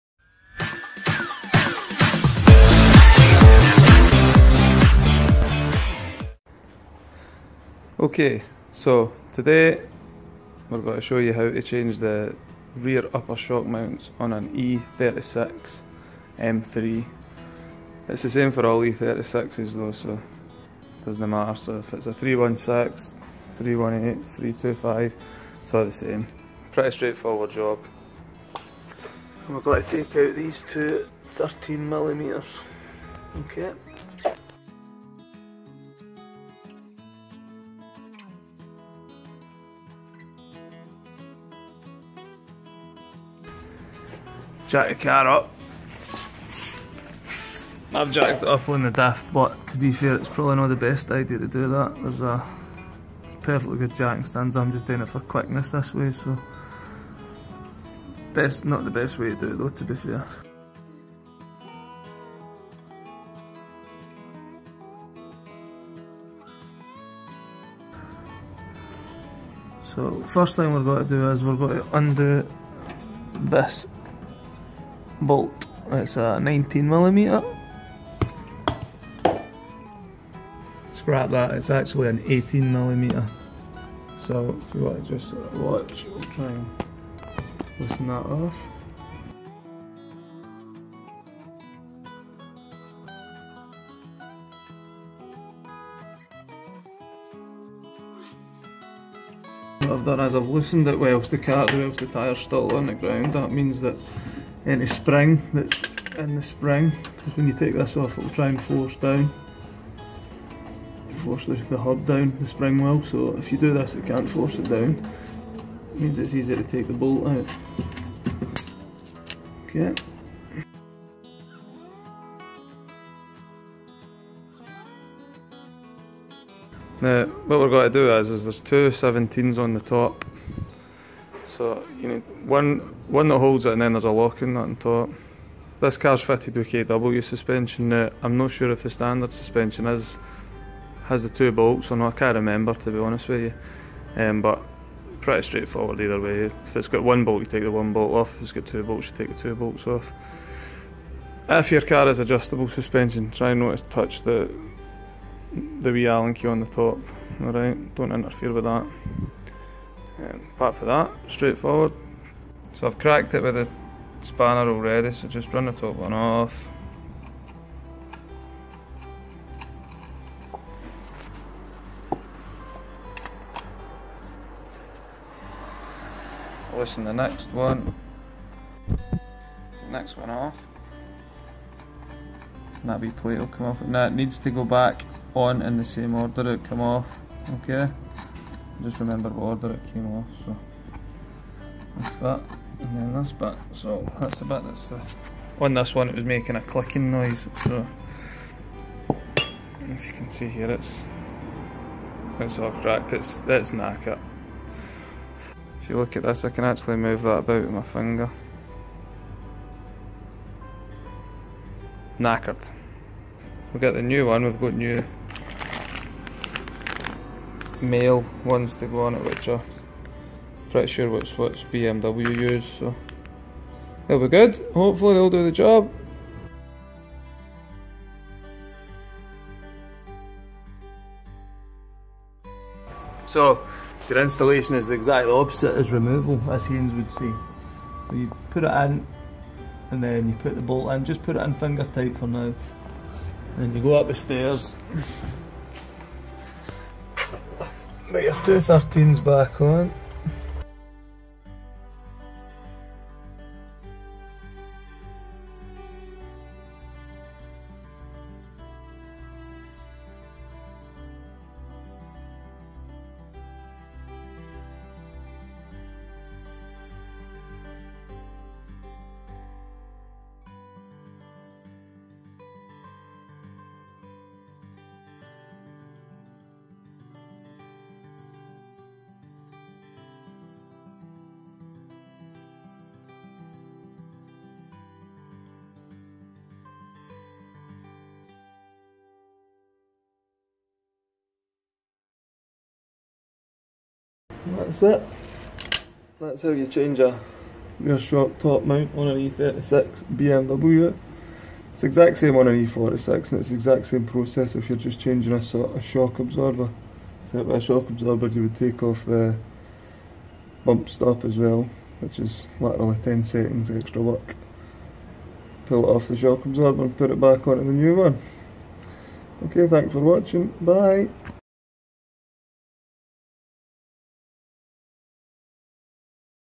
Changing my BMW rear suspension shock absorber mount due to a clicking / creaking noise. E36 e46
Changing the rear top shock absorber mount on my old e36 m3 after it developed a weird clicking or cracking noise. It Actually sounded like something dangling and hitting something now and again. Weird. Anyway it's fixed now 😊